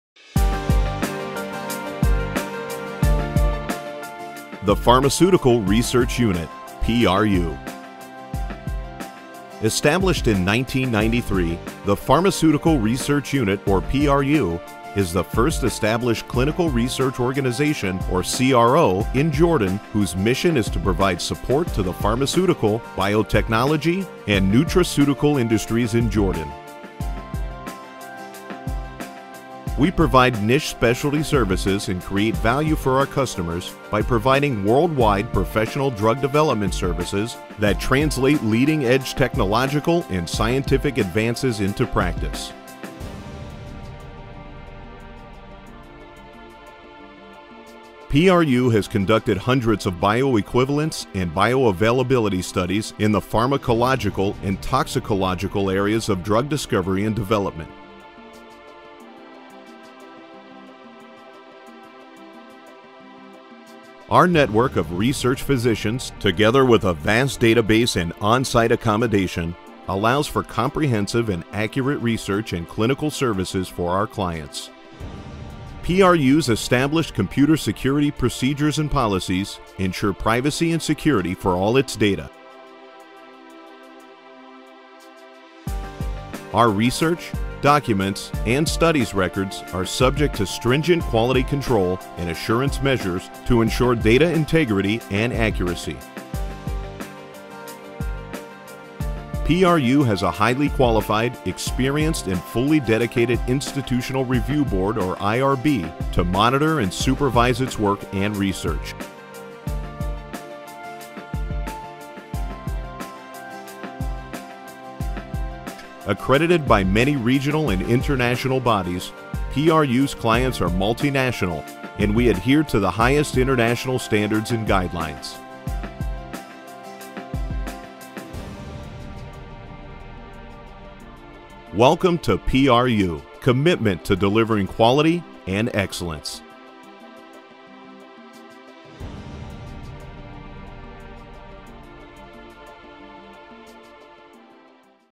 The Pharmaceutical Research Unity PRU Amman, Jordan
Established in 1993, the Pharmaceutical Research Unit (PRU) is the first established Clinical Research Organization (CRO) in Jordan whose mission is to provide support to the pharmaceutical, biotechnology and nutraceutical industries in Jordan. We provide niche specialty services and create value for our customers by providing worldwide professional drug development services that translate leading edge technological and scientific advances into practice.